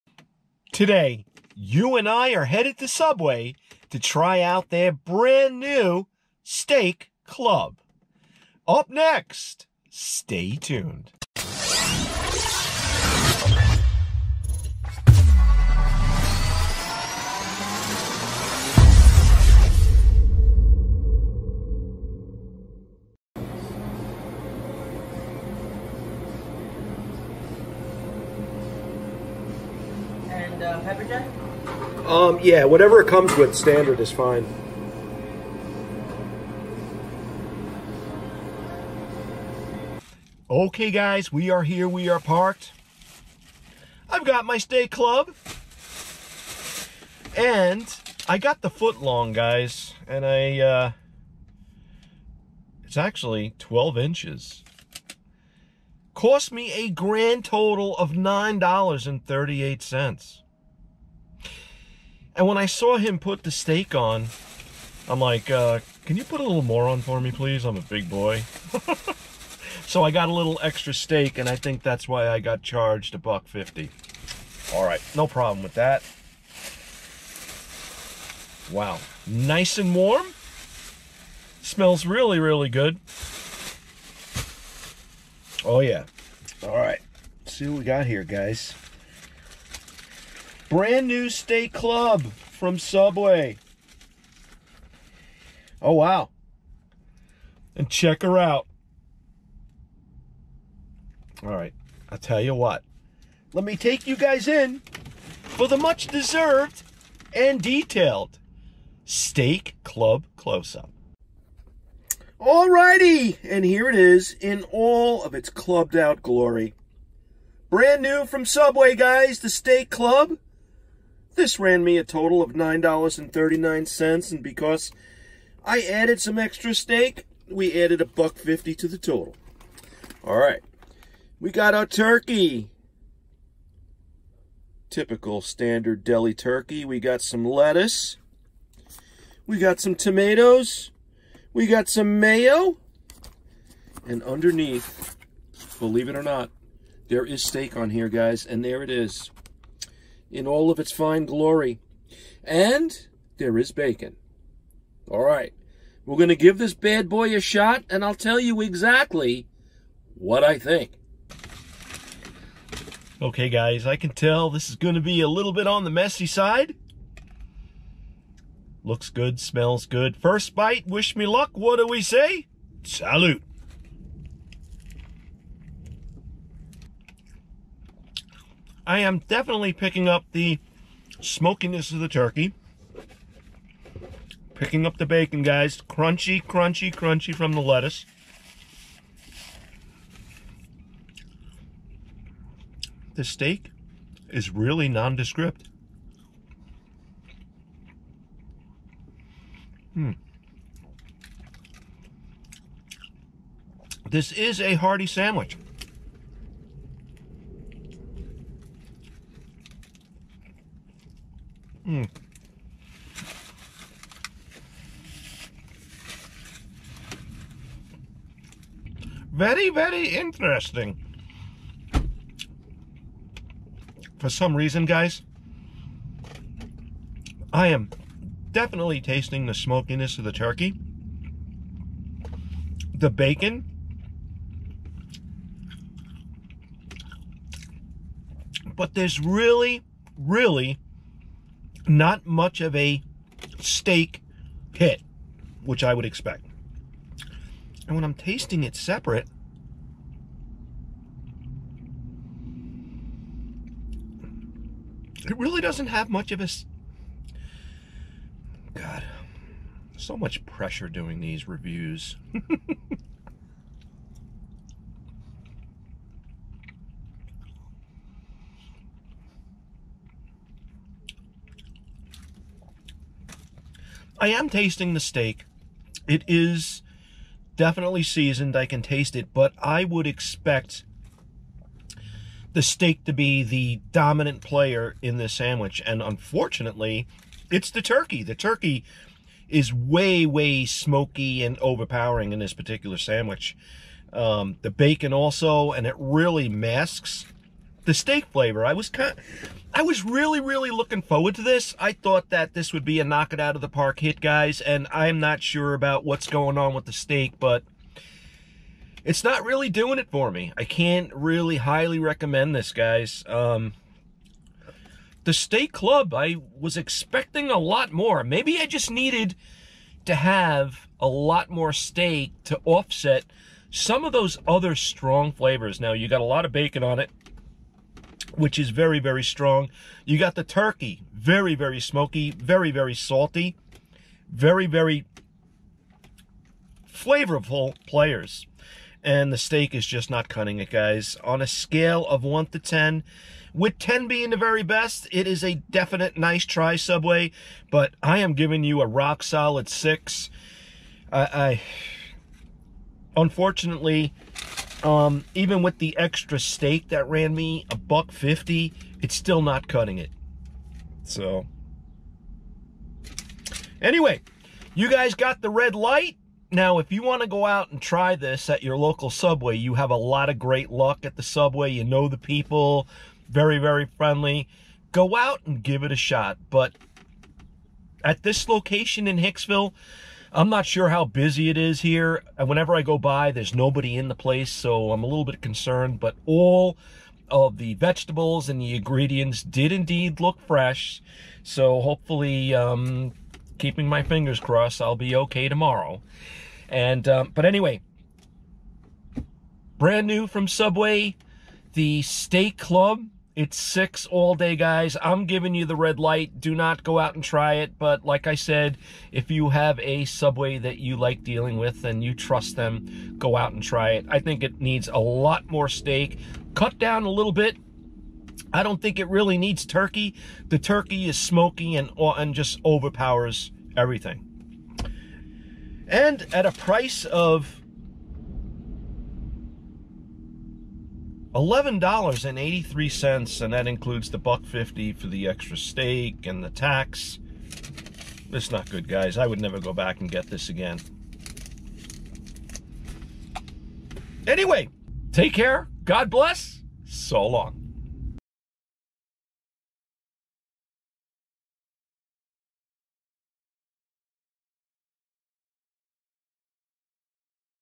Subway® | Steak Club 🥩 🧀 | Taste Test & Review 😀 | JKMCraveTV
In this video I try the Subway® Steak Club which cost me a total of $11.39. Is this something you would want to eat? What to find out my take on the steak club.

Make sure to CLICK the 🔔 and you will NEVER MISS A THING!

Joe Mueller, Food Reviewer JKMCraveTV